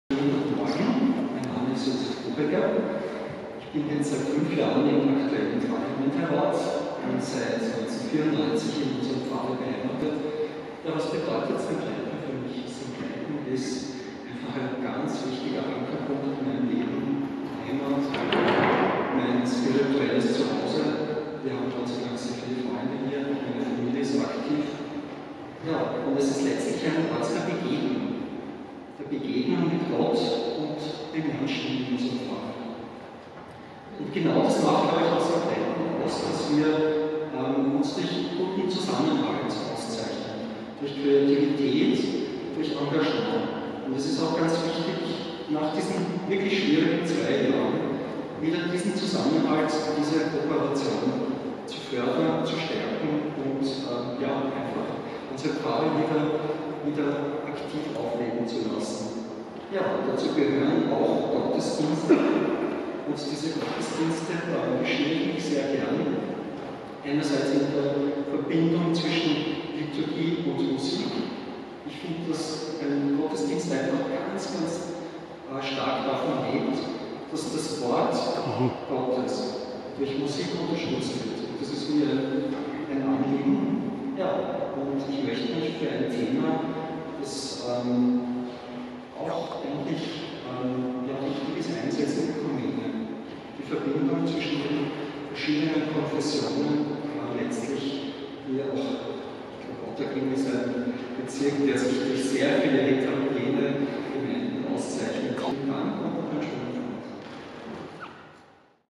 Vorstellung Josef Buchecker für PGR Wahl 2022
Josef Buchecker stellt sich in der Messe am 6.März als Kandidat für die Pfarrgemeinderatswahlen vor, die am 20. März 2022 stattfinden.